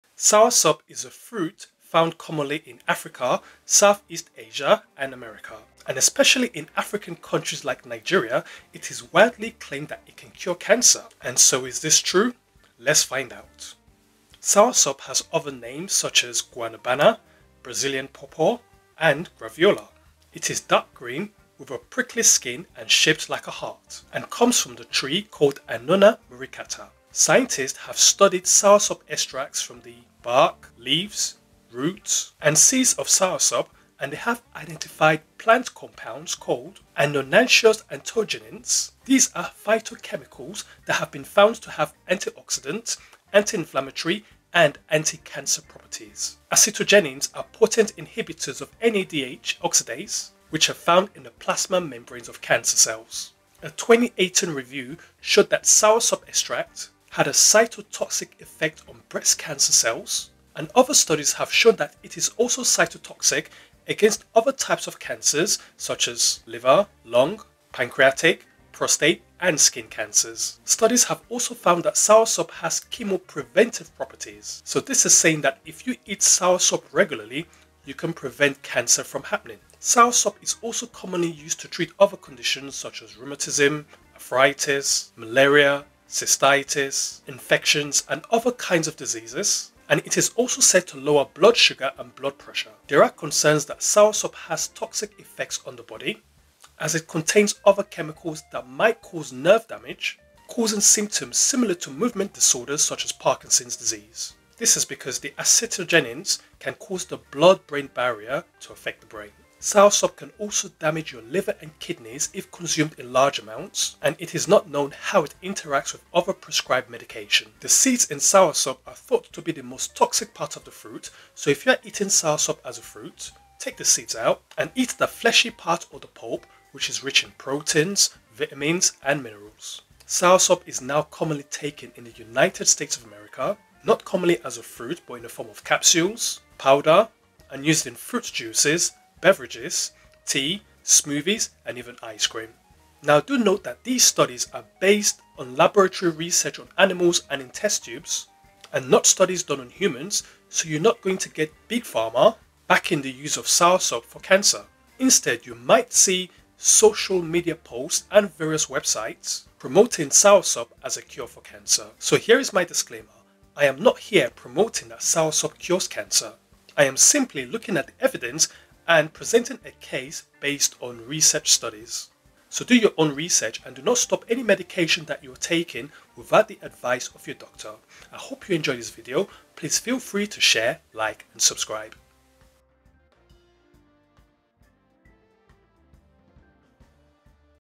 How Soursop Helps To Fight Cancer | Health Benefits And Risks of Soursop (Graviola)
Soursop is also know as Graviola, Guanabana, and brazilian pawpaw. It is a dark green fruit, with a prickly skin and shaped like a heart, from the tree called Annona Muricata.
Soursop extracts contains annonaceous acetogenins, phytochemicals with antioxidant, antiinflammatory and anticancer properties. Acetogenins are potent inhibitors of NADH oxidase (nicotinamide adenine dinucleotide phosphate-oxidase) of the plasma membranes of cancer cells.
A 2018 review showed that soursop extracts had cytotoxic effect on breast cancer cells, and other studies have shown that it is also cytotoxic against other cancers such as pancreatic, lung, liver, prostate and skin cancers. Studies also found that soursop has chemopreventive properties against cancer. Other Soursop health benefits include treating arthritis, rhuematism, malaria, cystitis, infections and other kinds of diseases and it is also said to lower blood sugar and blood pressure.
Risks of soursop include nerve damage causing movement disorders like Parkinsons disease, liver and kidney damage.

**Anticancer Properties of Graviola (*Annona muricata*): A Comprehensive Mechanistic Review - 2018**

A review on a miracle fruits of Annona muricata - 2015